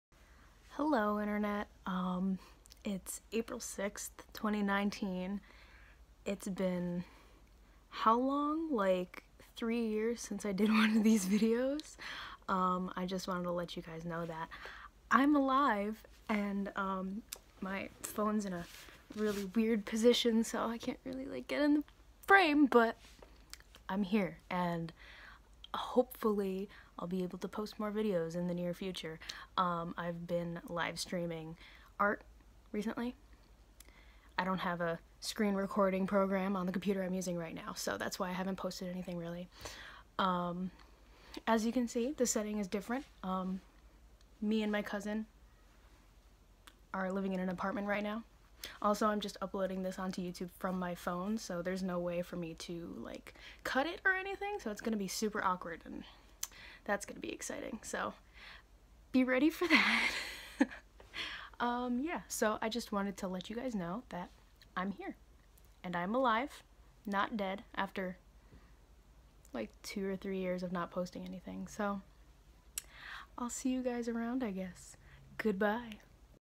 Tsune’s Not-So-Daily Vlog 4-6-19
Howdy, internet peeps! :D

I’ve been absent on here, and here’s a video explaining why, sort of! I thought I’d give a little hello since I’m home alone and bored lmao. XD

The name is changing because my sona’s name has changed! Lots of other things are changing as well. We’ll get to that later. :3c

For those of you who have kept me on your subscriber list for all this time, thank you!! I really appreciate your patience, and I promise I’ll be back soon!